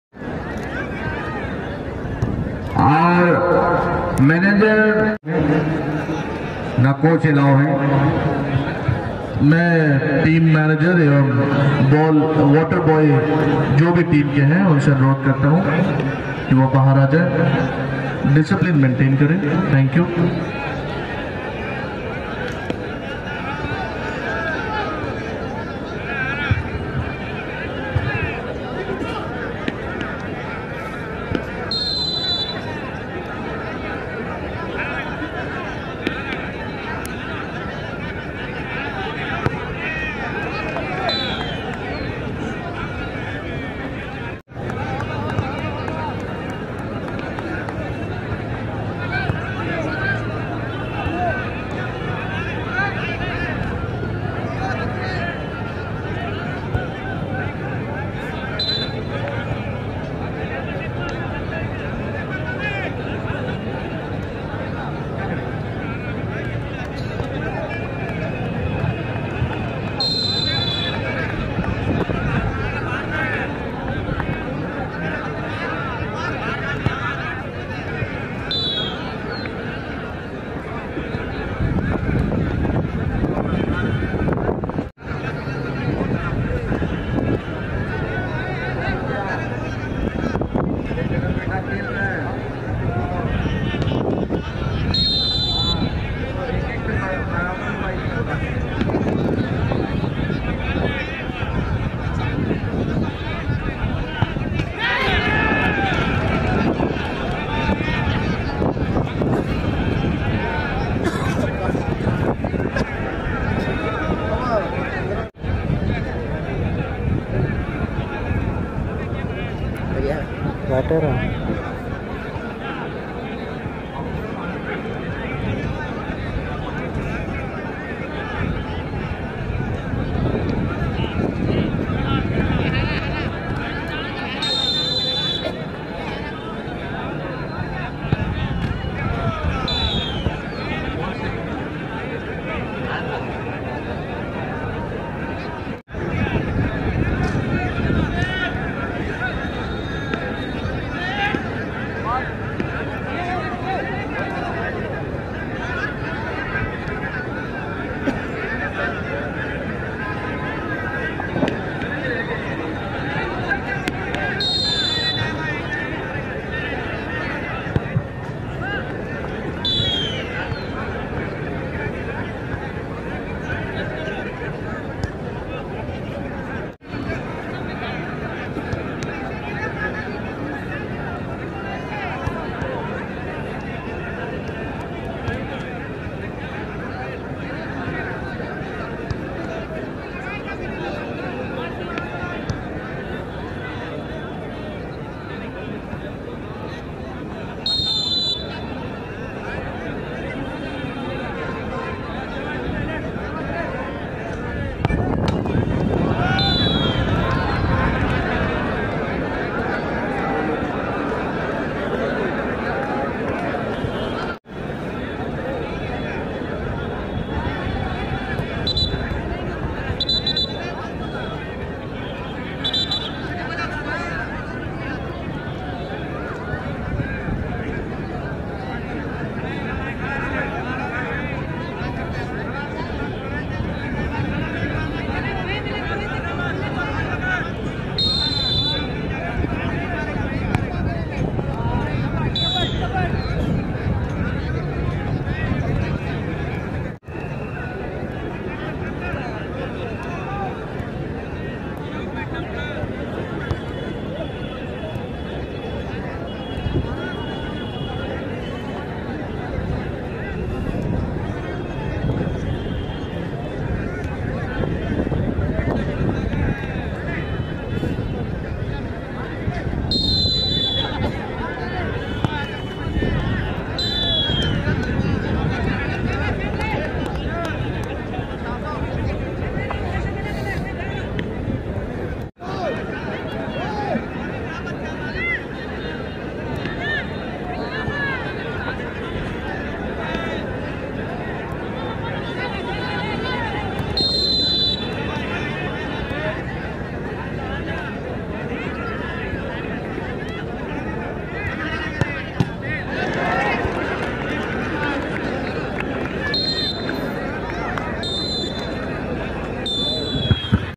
Lion Heart (Kingfisher FC) 🆚 Om Namah || 1st Round Penalty Kick || Arjuna Trophy Jharkhand 2023
Organized by :-  ARJUNA TROPHY TELCO FOOTBALL JHARKHAND 2023 _________________________________________
 Kingfisher FC Potka
           v/s
 Om Namah

Best ⚽️ Match Jharkhand, Odisha

FULL SUPPORT :
SIDESHWAR HEMBROM
CHINTA BODRA
GANESH BIRULY
DEVKANT BAGE
PRADHAN KALA
PHILIP GANDI _________________________________________